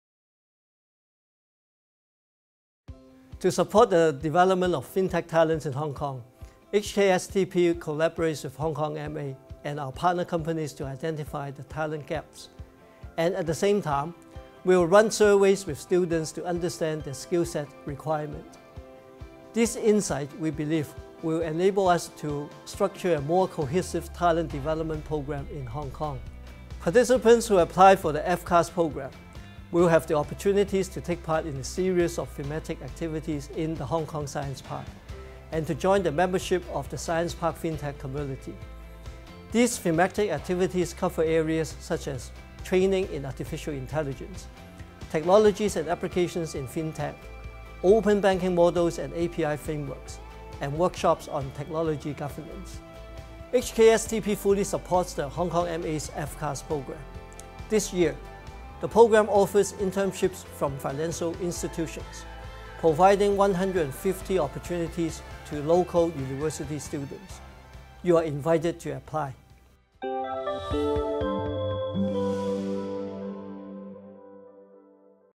香港金融管理局金融科技人才培育計劃 – 香港科技園公司金融科技社群(Fintech Community)
香港金融管理局金融科技人才培育計劃(FCAS) 2020/21截止報名日期延長至2020年4月10日，有興趣參加者報名從速! 香港科技園公司為促使金融科技生態圈內不同持份者有更多互動交流，將會舉行專題活動，以建立一充滿活力金融科技社群。香港科技園公司全力支持是次計劃，金融科技社群相關活動將開放予FCAS參加者參與，與金融及創科界精英互相聯繫，建立網絡。

「金融科技人才計劃2020/21」截止報名日期: 2020年4月10日